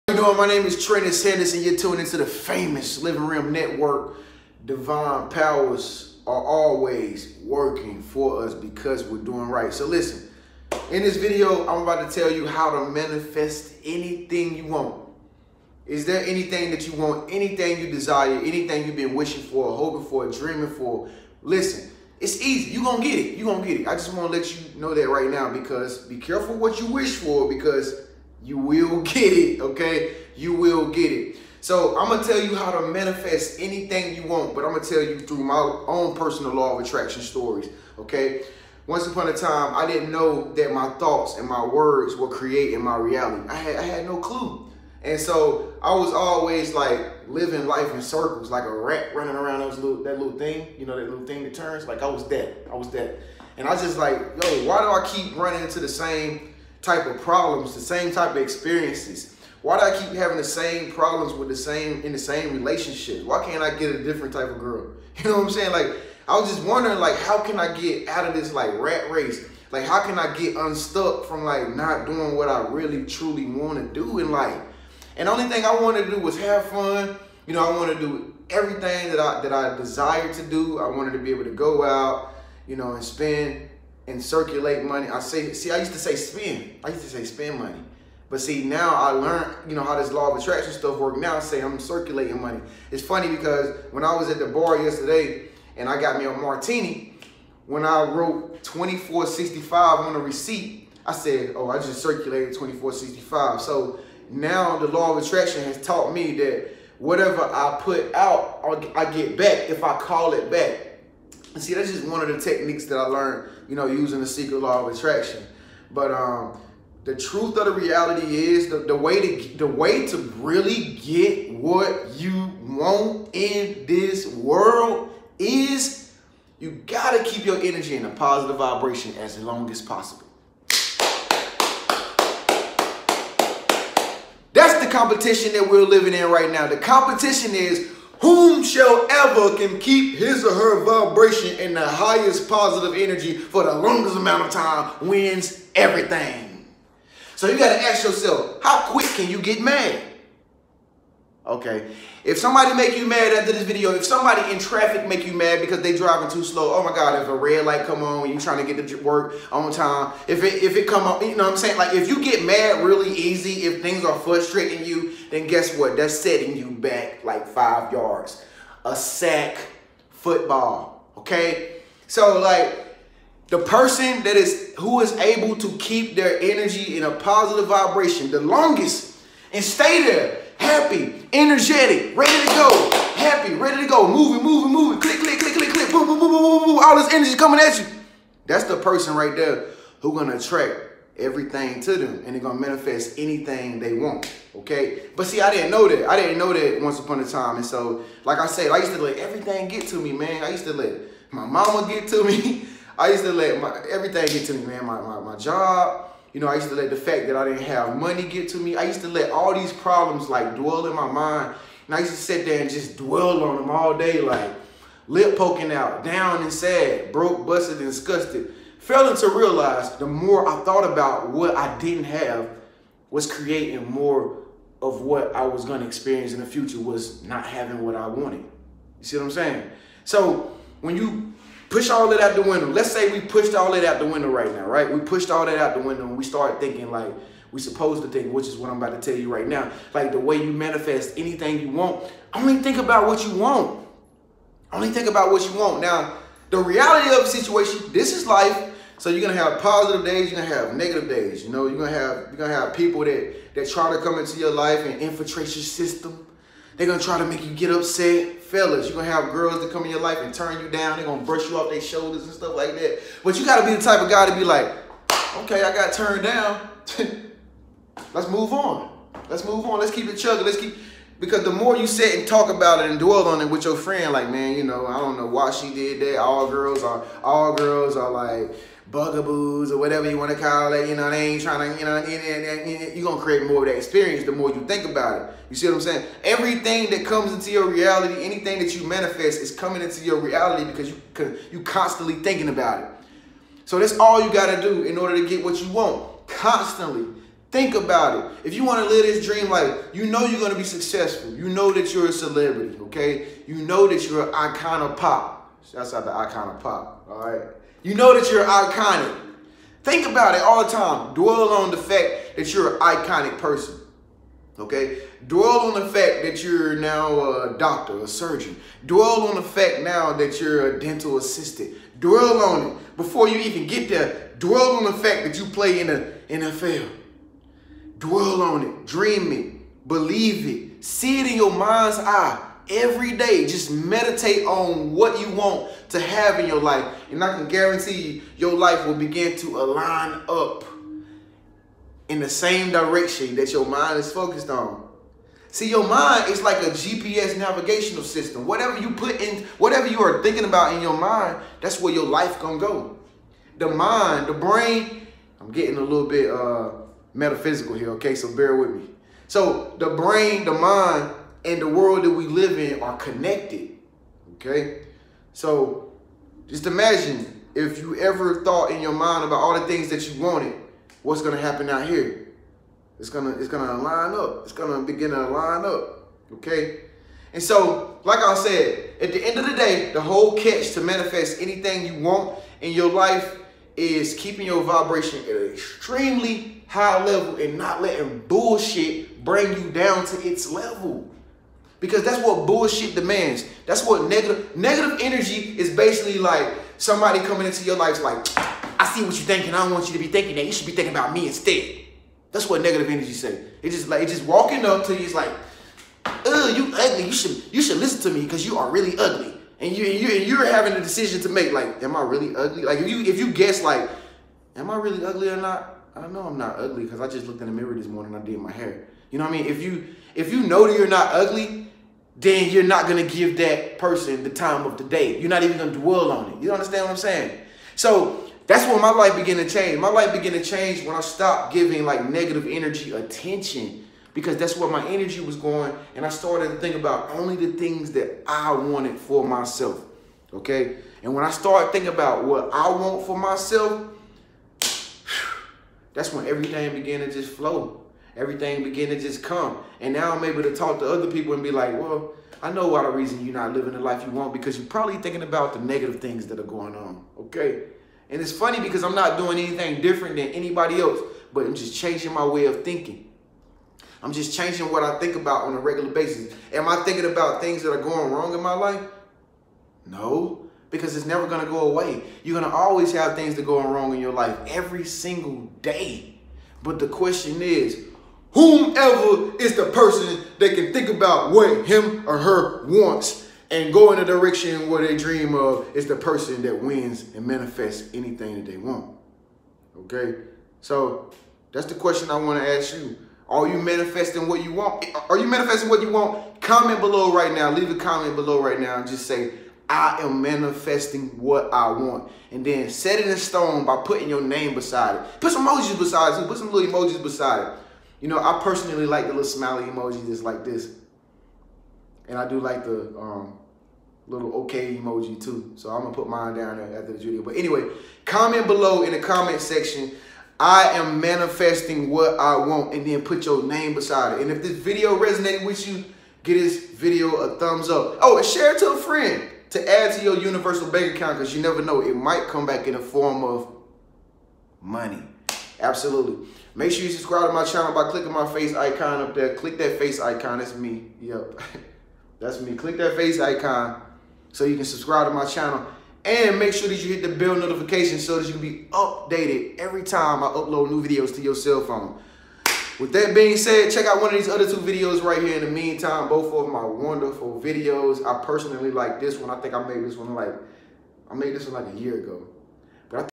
How to Manifest ABSOLUTELY ANYTHING!! + MANIFESTATION STORY!!
My manifestation story will help YOU manifest absolutely ANYTHING you want using the Secret Law of Attraction!

▼BOOST YOUR YOUTUBE CHANNEL NOW!

My Favorite Book I Recommend

EQUIPMENT:
Camera: Nikon DSLR D3200
Editing Software